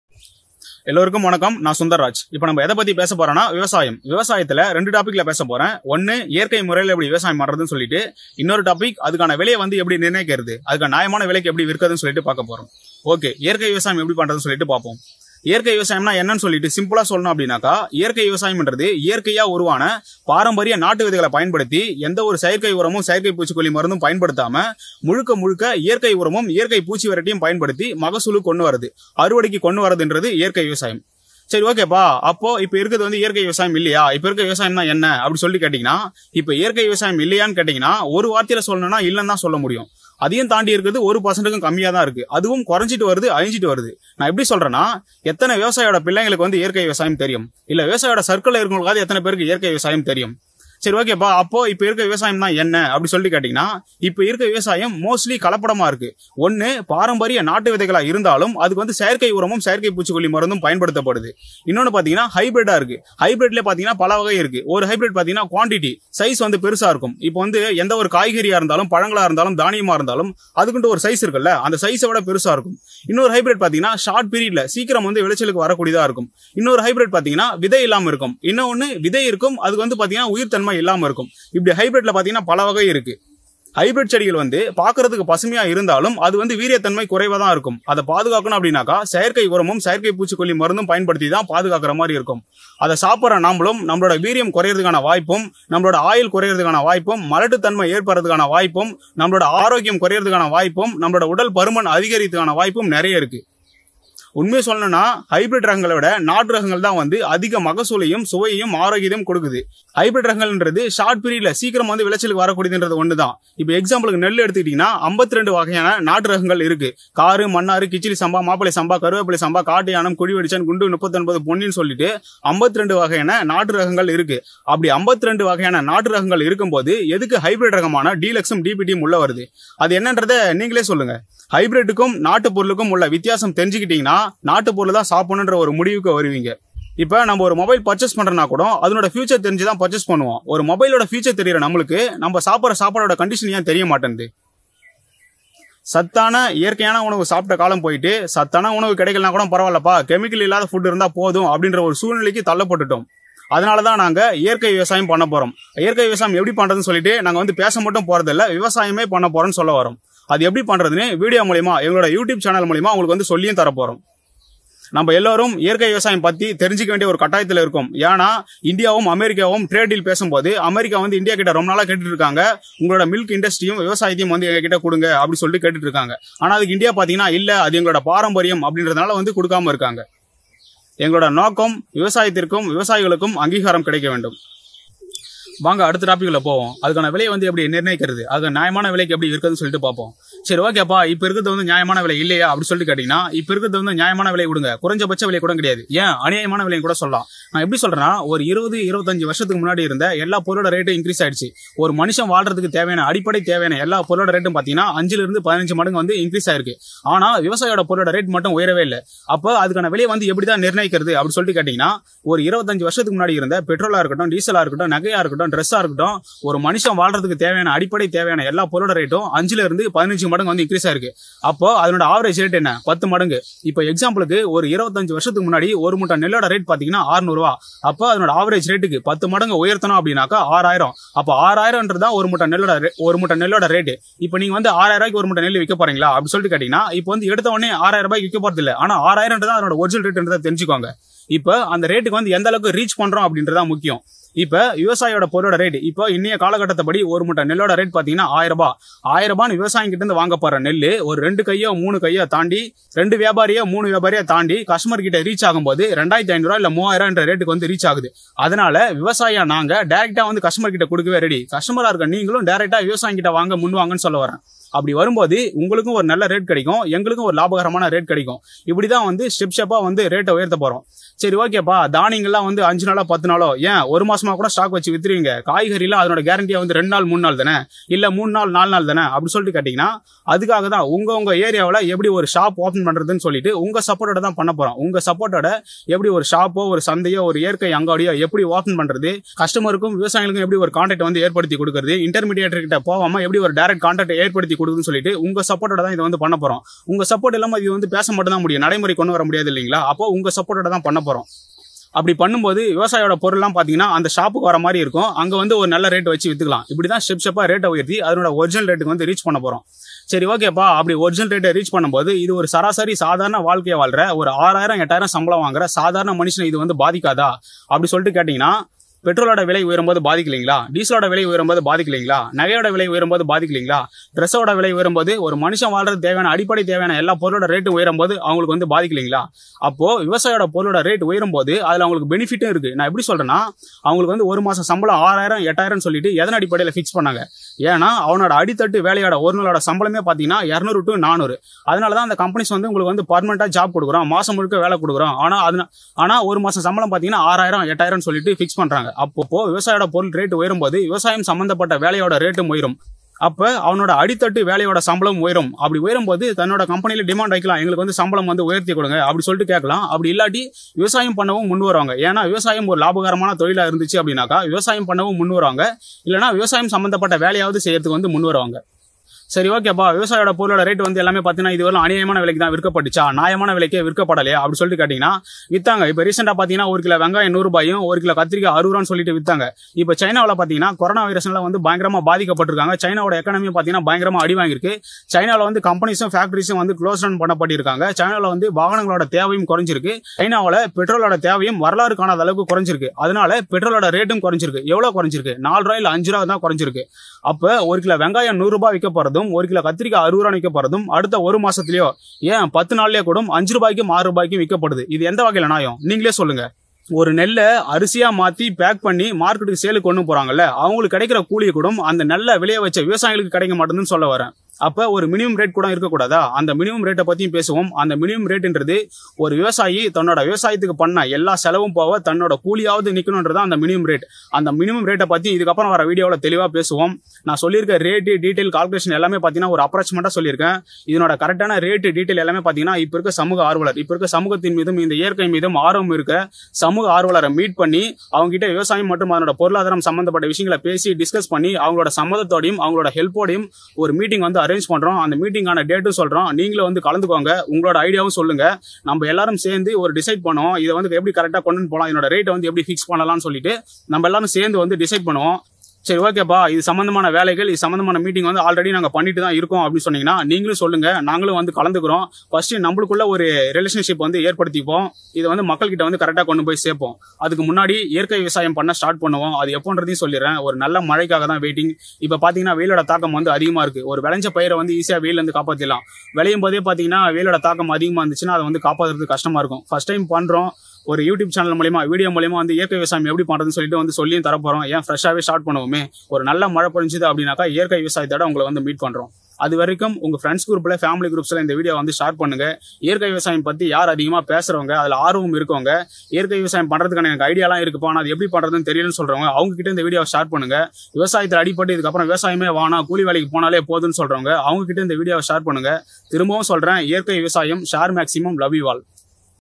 Agriculture and Farmers Should be Recognized | விவசாயிகளுக்கு அங்கிகாரம் கிடைக்க வேண்டும்.
How to naturalize farming and how to determine its price.
இயற்கை முறையில் எப்படி விவசாயம் செய்வது, விலை எப்படி நிர்ணயம் செய்வது' நியாய விலைக்கு எவ்வாறு விற்பது.

Agriculture and it's Economy
விவசாயம் மற்றும் அதன் பொருளாதாரம்

The food we eat becomes adulterated and the chemical is mixed
நாம் சாப்பிடும் உணவு கலப்படம் ஆனது மற்றும் ரசாயனம் கலந்து

Know the difference between natural food and hybrid food

How to farm in a natural way

The food you eat is adulterate. ?

We all need to know about natural agriculture because when INDIA and the US talk about trade, the US tells INDIA your agriculture.